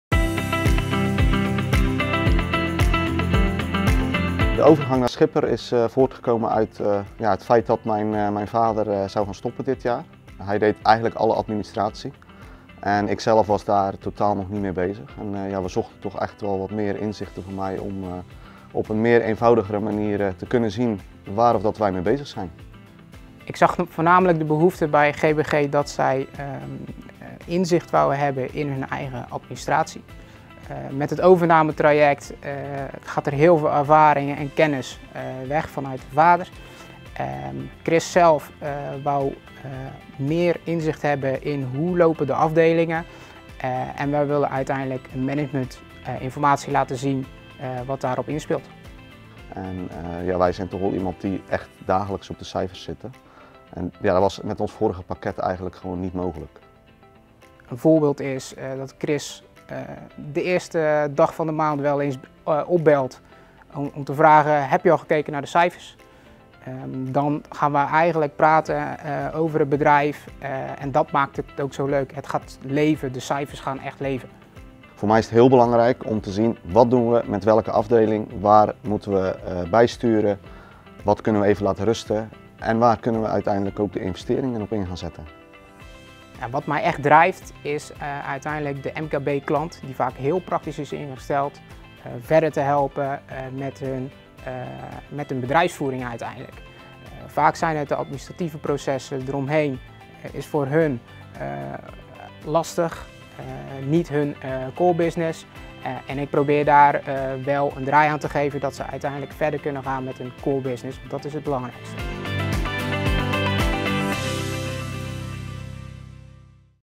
Samenwerking tussen Schipper Accountants en GBG
"Het vertrouwen dat ik direct bij de eerste kennismaking al had, bleek terecht te zijn.  Maurice en zijn collega’s hebben heel goed begrepen wat ik wil. Met het financiële dashboard dat speciaal voor mij is ingericht, zie ik in één oogopslag hoe we ervoor staan", vertelt ondernemer Chris van Antwerpen van GBG.